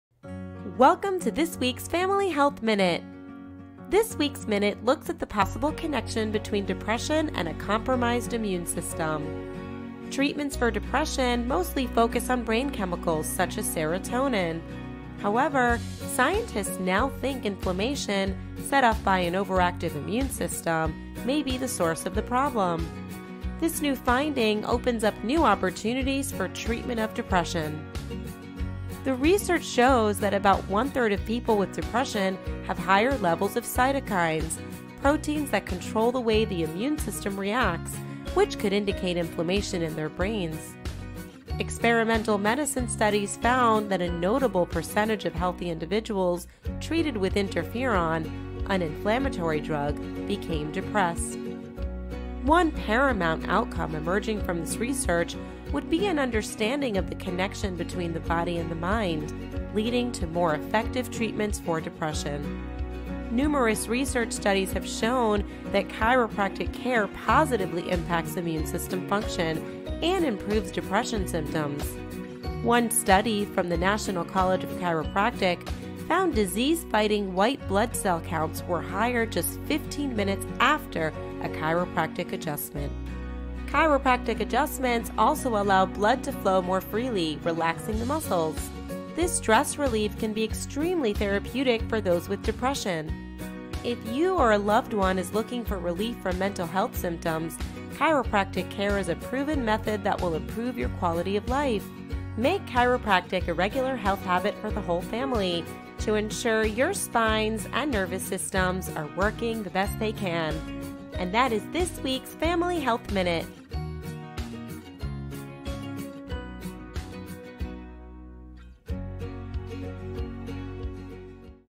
Depression and the Immune System
This week’s minute looks at the possible connection between depression and a compromised immune system.

Treatments for depression mostly focus on brain chemicals, such as serotonin; however, scientists now think inflammation, set off by an overactive immune system, may be the source of the problem.

This new finding opens up new opportunities for treatment of depression.

The research shows that about one-third of people with depression have higher levels of cytokines, proteins that control the way the immune system reacts, which could indicate inflammation in their brains.

Experimental medicine studies found that a notable percentage of healthy individuals treated with interferon, an inflammatory drug, became depressed.

One paramount outcome emerging from this research would be an understanding of the connection between the body and the mind, leading to more effective treatments for depression.

Numerous research studies have shown that chiropractic care positively impacts immune system function and improves depression symptoms.  One study, from the National College of Chiropractic, found disease-fighting white blood cell counts were higher just 15 minutes after a chiropractic adjustment.

Chiropractic adjustments also allow blood to flow more freely, relaxing the muscles. This stress relief can be extremely therapeutic for those with depression.

If you or a loved one is looking for relief from mental health symptoms, chiropractic care is a proven method that will improve your quality of life.

Make chiropractic a regular health habit for the whole family to ensure your spines and nervous systems are working the best they can.

And that is this week’s Family Health Minute.

To get your health and life back,
Dr Nicole Bernic
Fourways Chiropractic
51 Galloway Avenue
Douglasdale